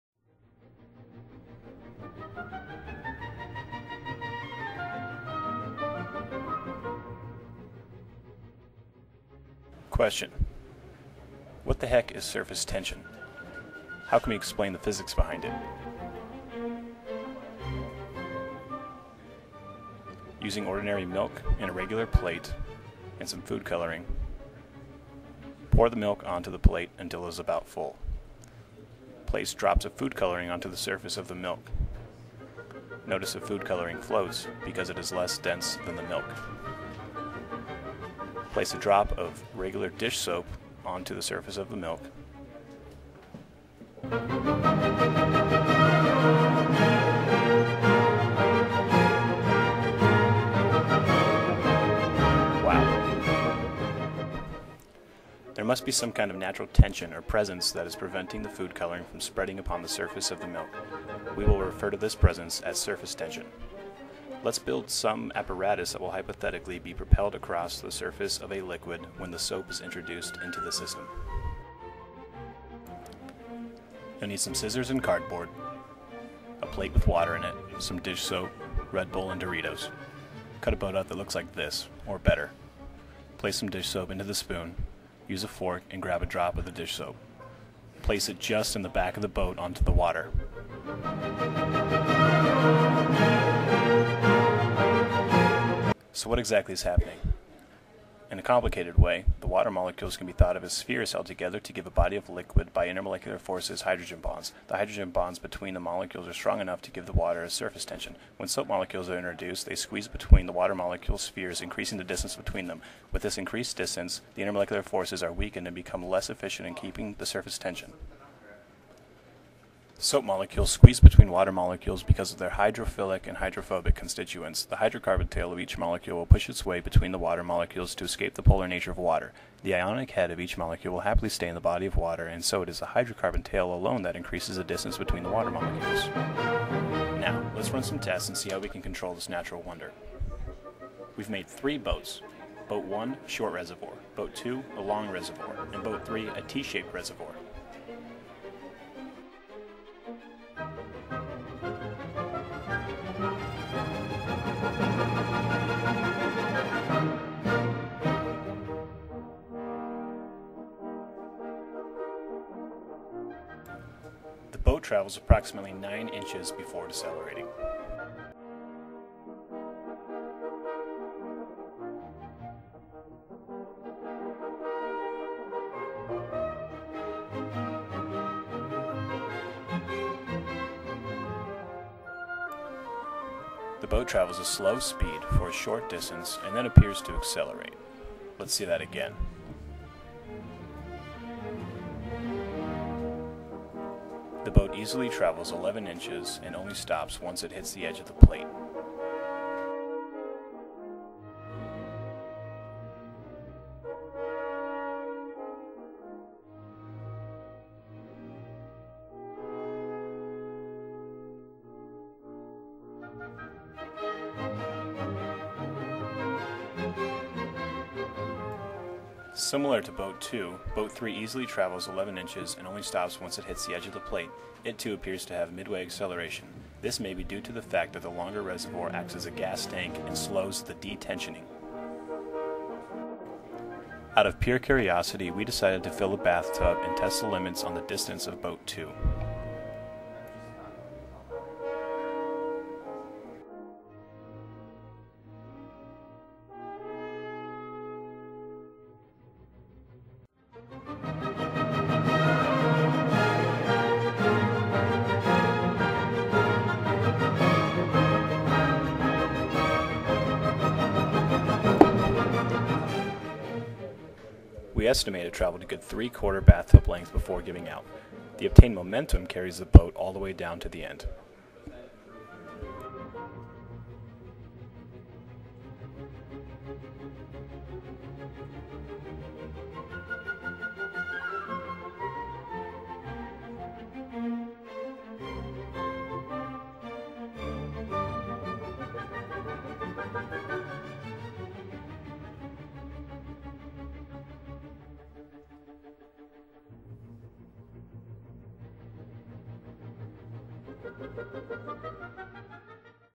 Boats and Soap - Surface Tension
Boats and Soap. Surface Tension at work. Prepare to be amazed... and stuff.